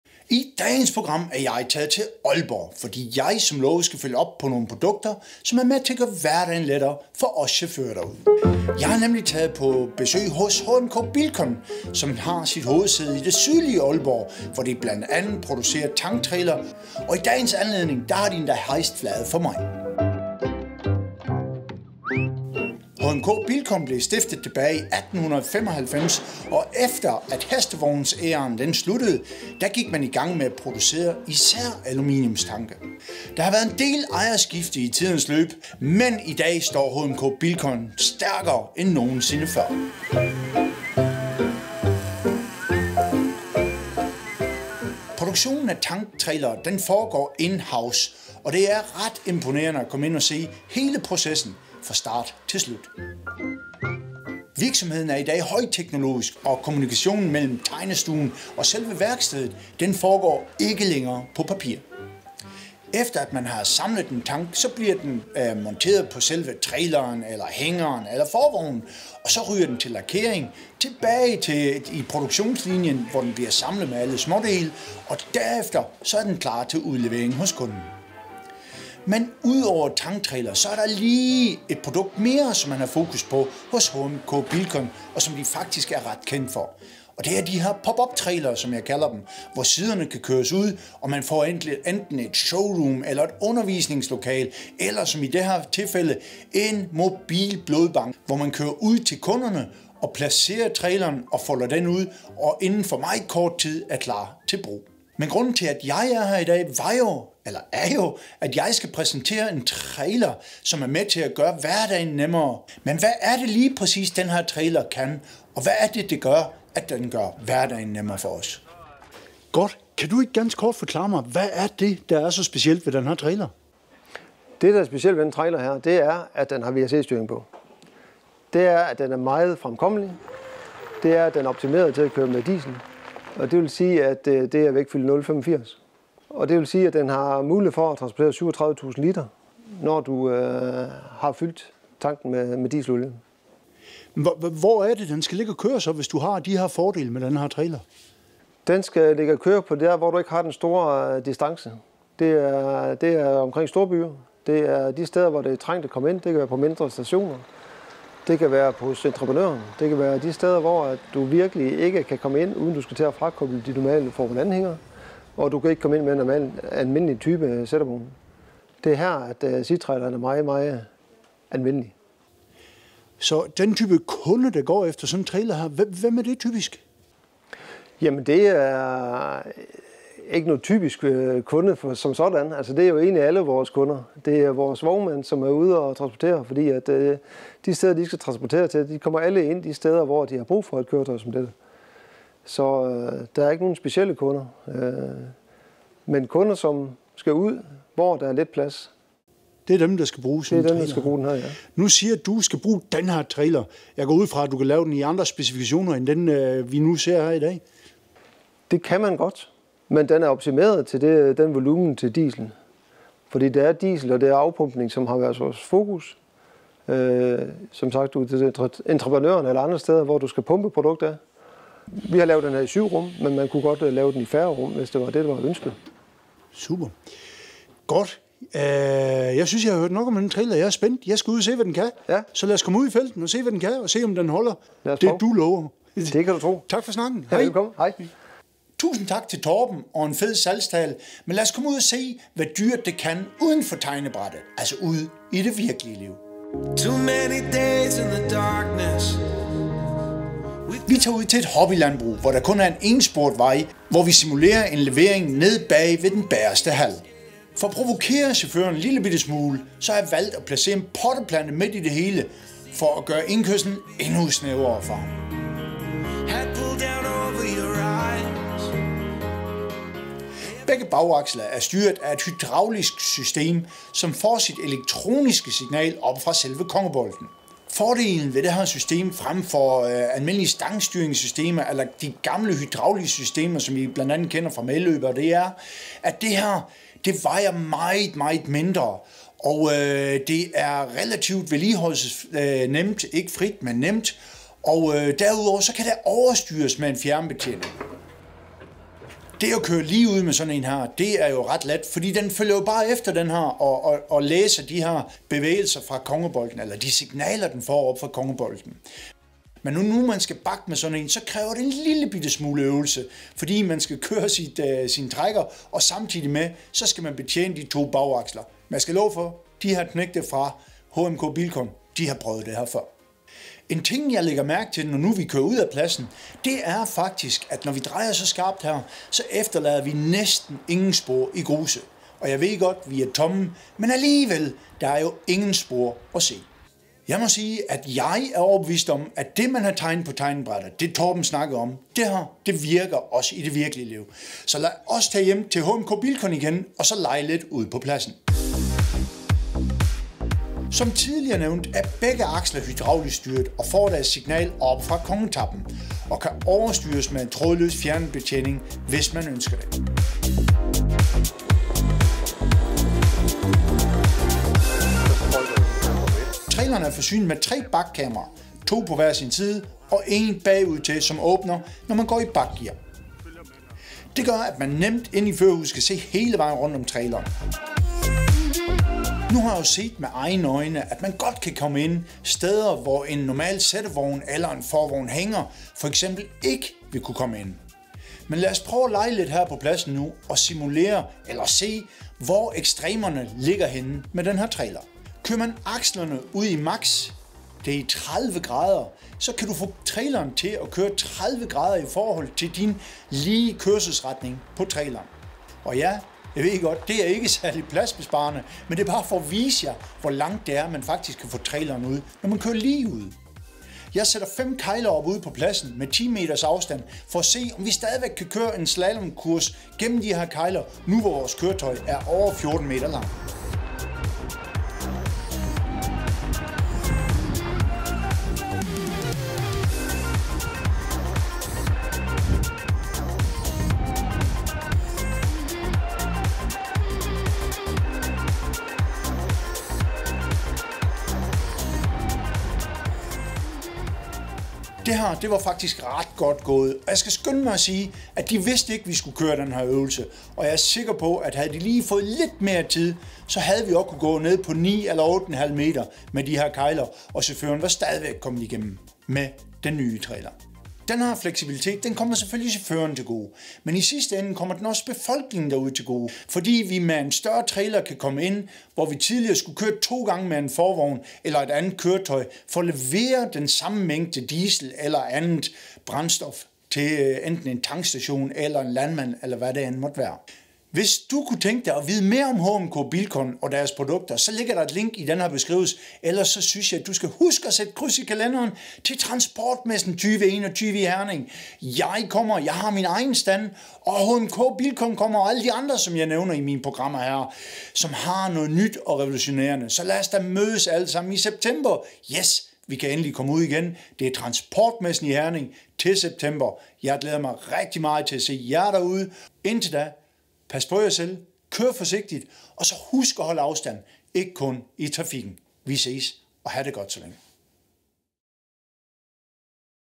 Ny TANK trailer
I serien af afsnit omkring produkter der gør det lettere for chaufføren i hverdagen, er jeg taget til Aalborg for at besøge HMK BILCON, som hovedsageligt producerer tank trailere. Der er kommet frem med en ny type tank trailer i deres produktsortiment, som giver chauffører og vognmænd nye muligheder der ude.

Skulle du kunne tænke dig at få besøg af os og få lavet en film omkring din virksomhed, er du velkommen til at kontakte os under .